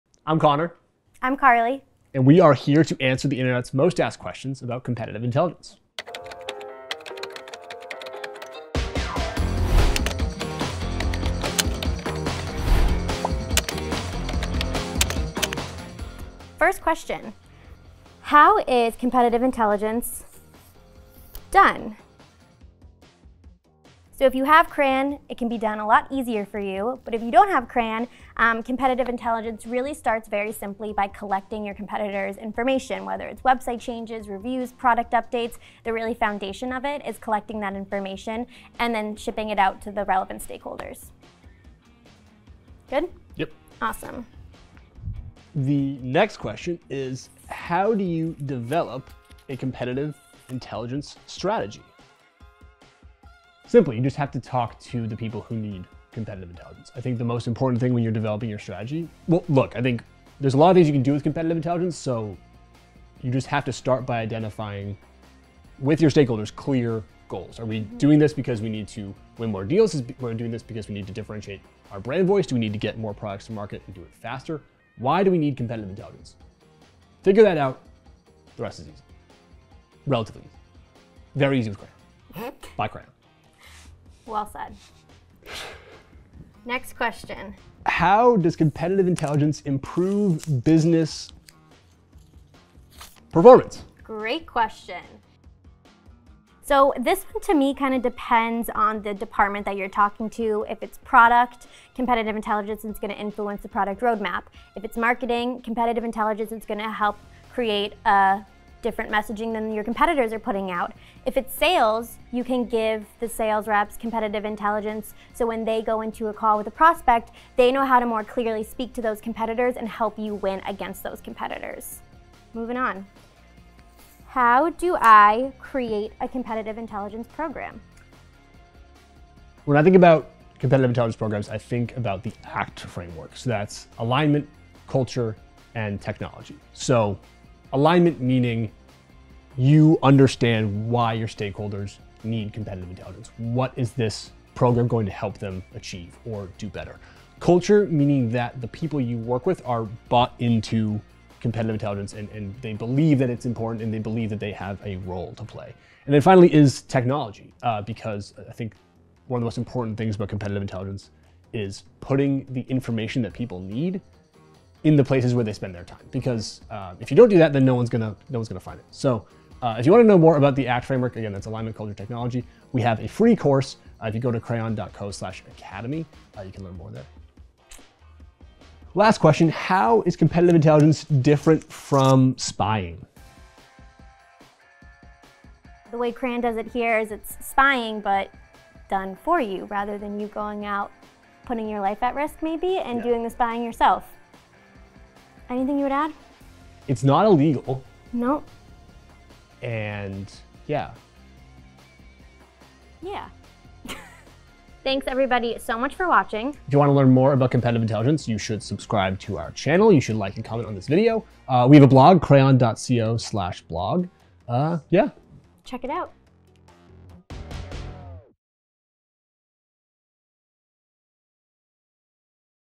Internet's Most Searched Questions About Competitive Intelligence Part 2
Sit down with Crayon's own Carly Cohan and Conor Bond as they answer the Internet's Most Searched Questions About Competitive Intelligence, Part Two: "What... competitive intelligence?"